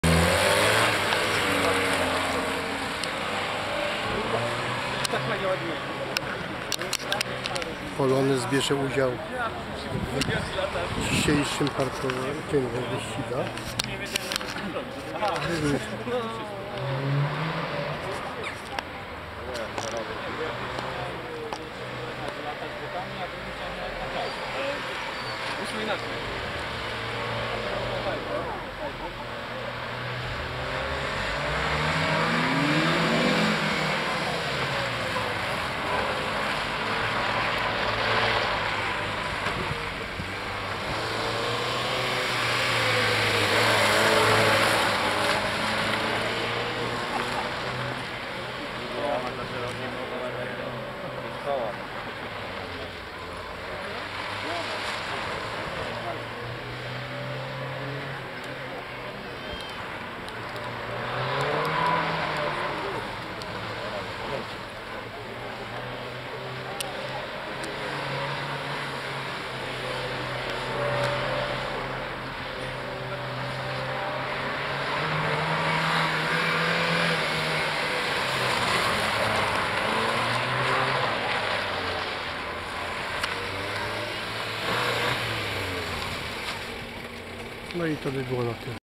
MVI 1806 POLSKA Sieradz Polonez tanczy na parkingu
Zawody wyscigow samochodowych...przy prokuraturze i ZUS w Sieradzu